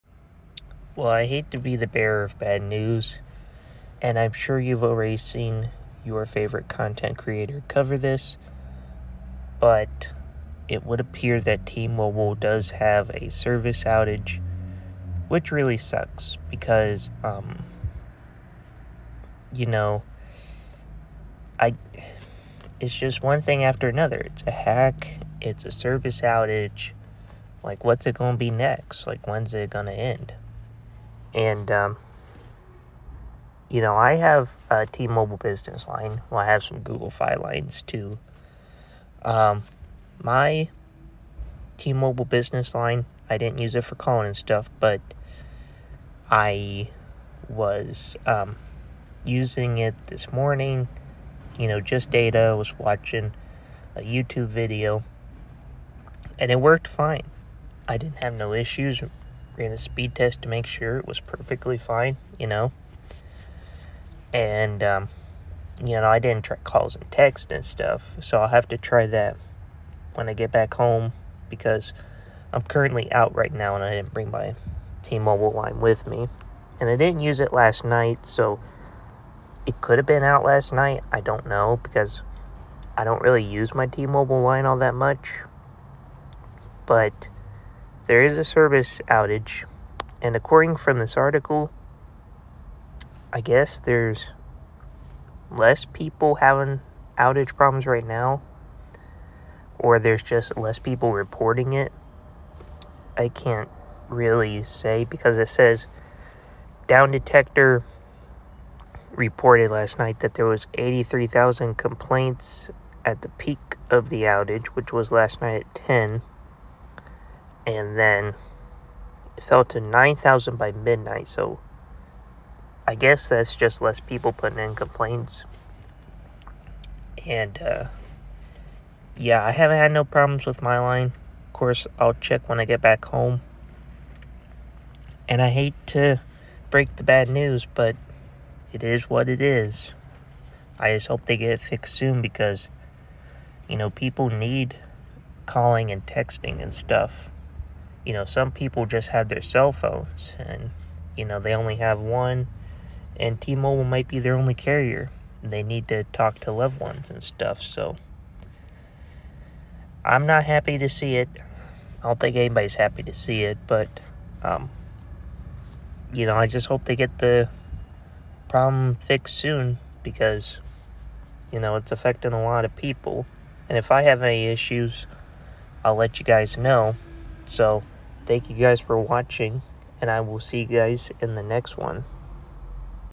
T-Mobile Nationwide Outage? Unbelievable 🤦🏼‍♂️
T-Mobile at it again. Has a service outage and blames it on anyone but them. I’m just baffled. The data on my business line worked fine earlier. But, I will check everything when I get back home to guarantee nothing else is wrong.

Thank you guys for watching. Sorry I had to break the bad news. If you didn’t already know.